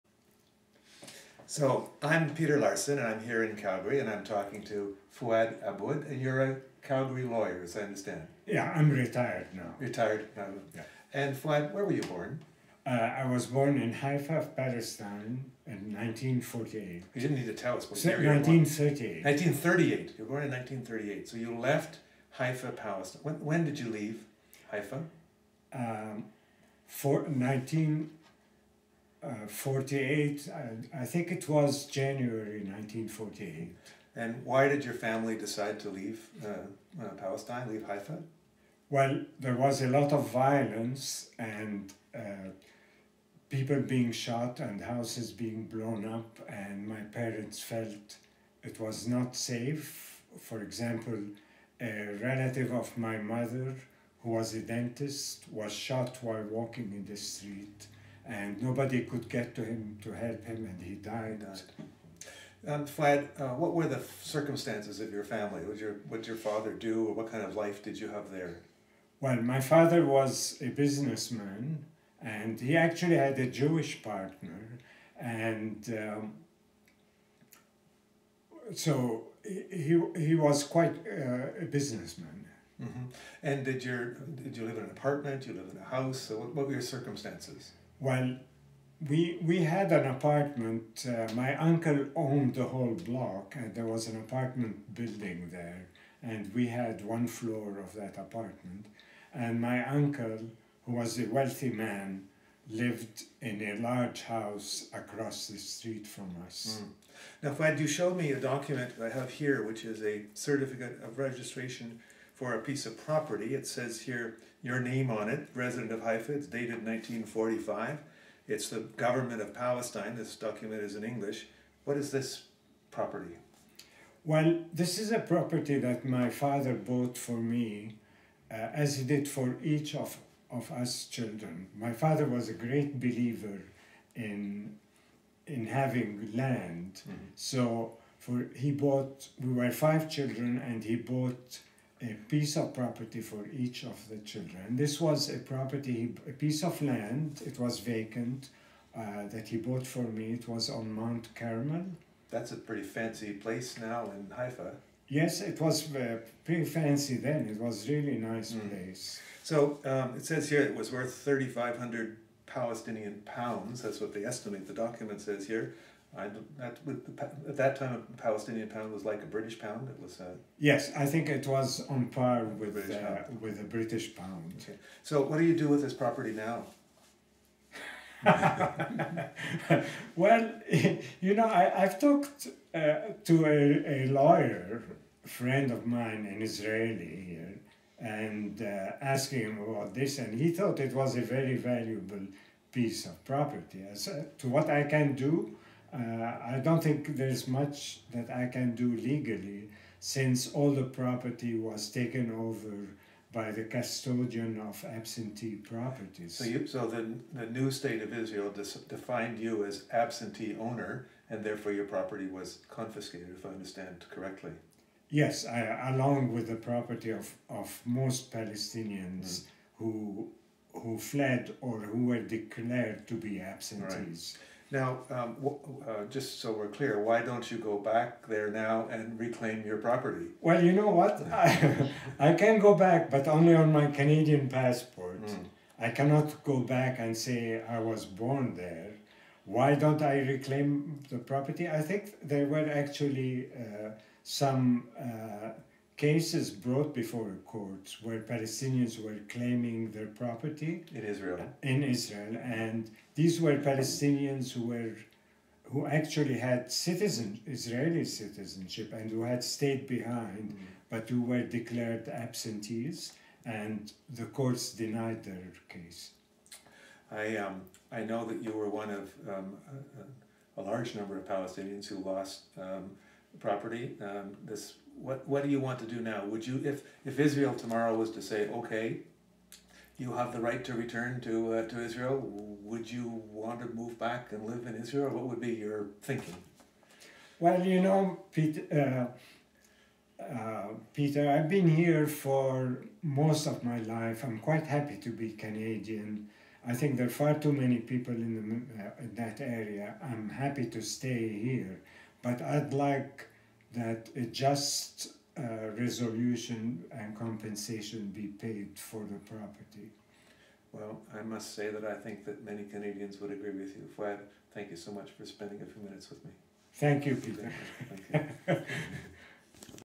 Interview Fuad Abboud dec 5 2016
Peter Larson, Chair of CTIP interviews Canadian lawyer Fuad Abboud, born in Haifa, Palestine 1938. He fled Palestine to Lebanon in 1948 during the Nakba.